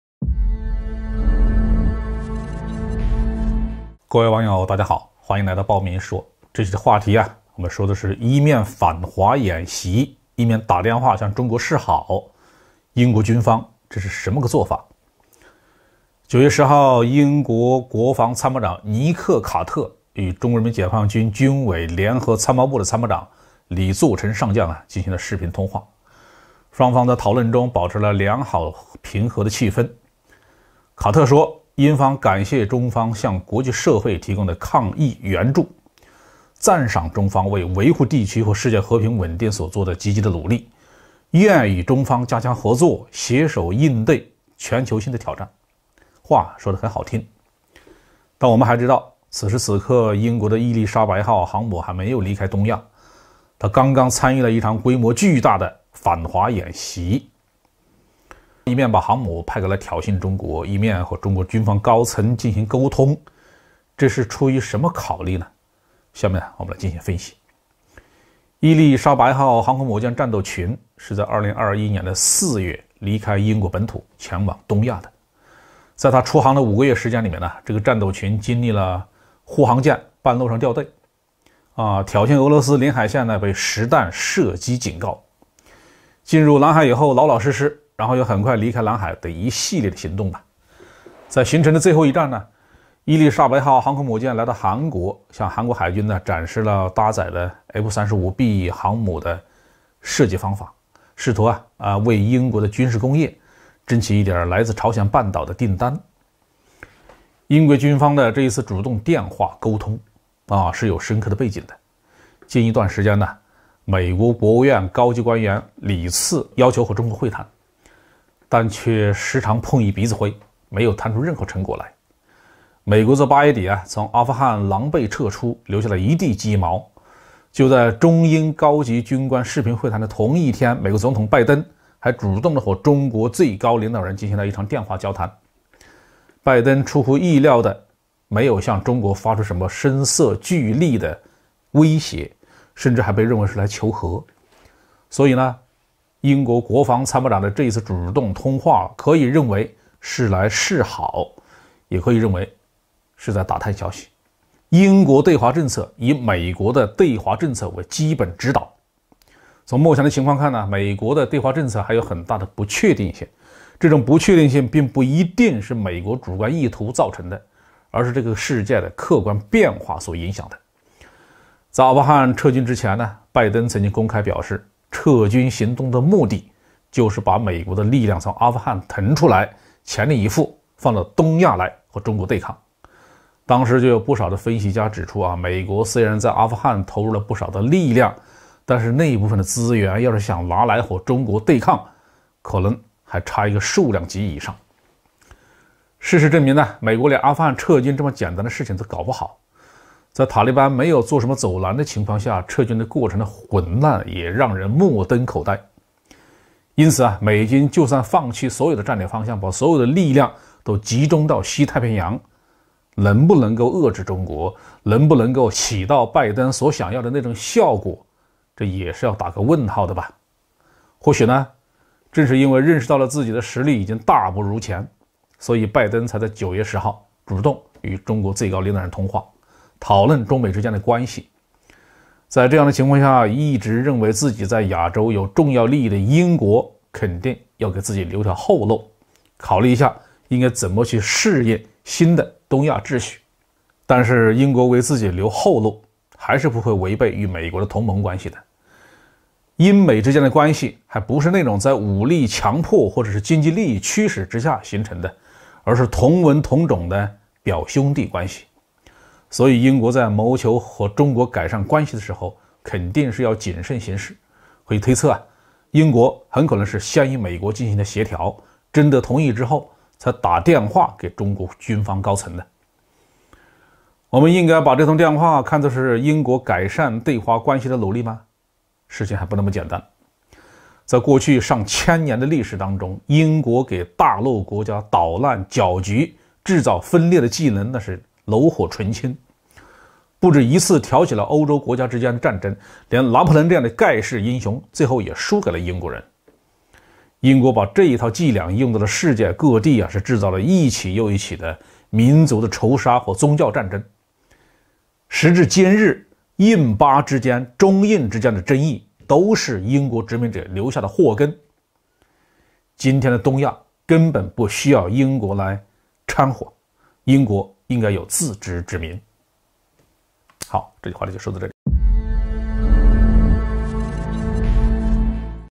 英航母還在反華演習，英軍高層卻打電話向中國示好，美日難以接受【包明大校】
一面反華演習一面打電話向中國示好，英國軍方這是什麼兩面派手法
英航母還在反華演習，英軍高層卻打電話向中國示好，美日難以接受